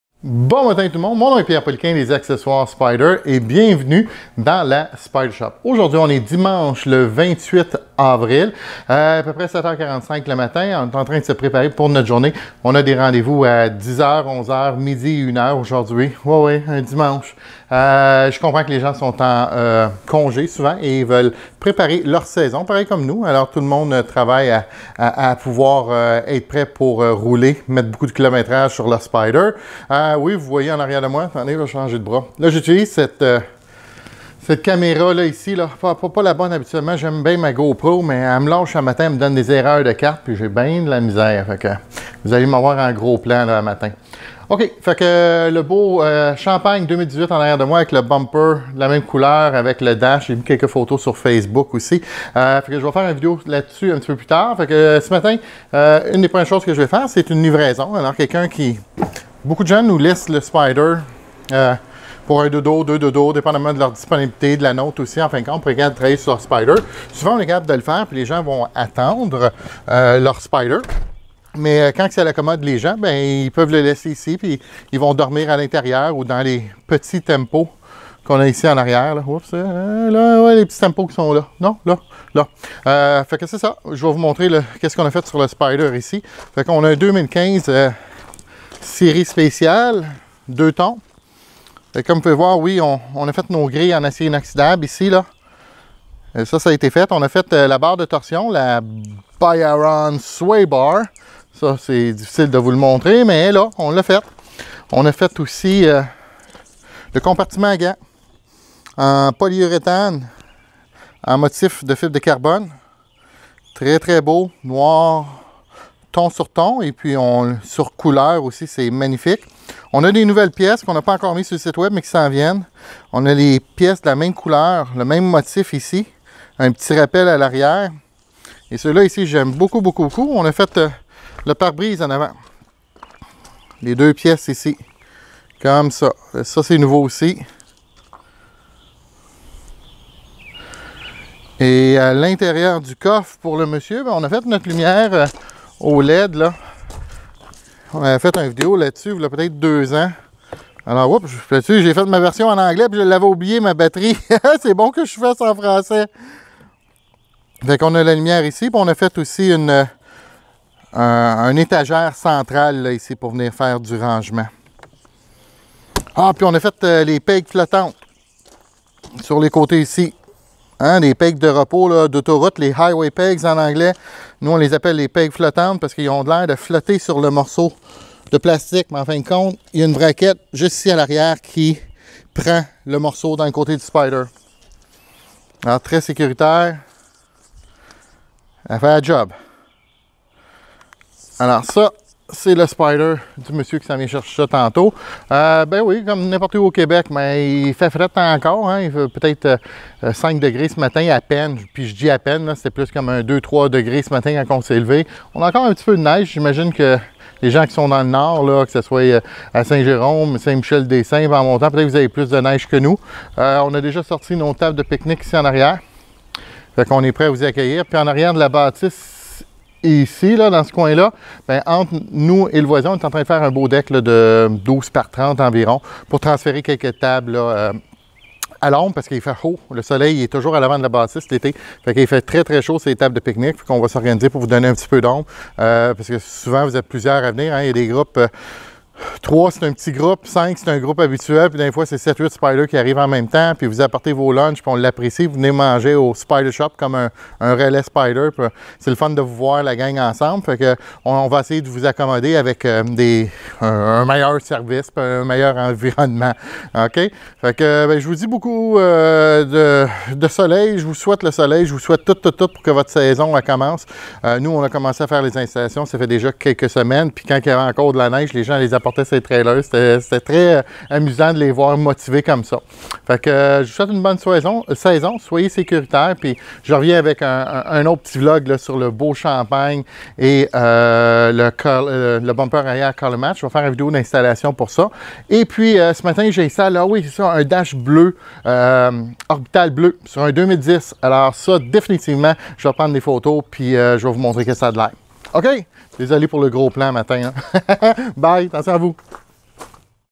Vlog 35 Français - The Spyder Shop - Ma journée et des modification sur un RT
Bon matin les amis Spyder,
Juste un aperçu de ma journée du dimanche, quelques modifications sur un Spyder de série RT.

Pierre Poliquin
917 Chemin Georges Lavaltrie
Québec Sur RDV svp. Merci.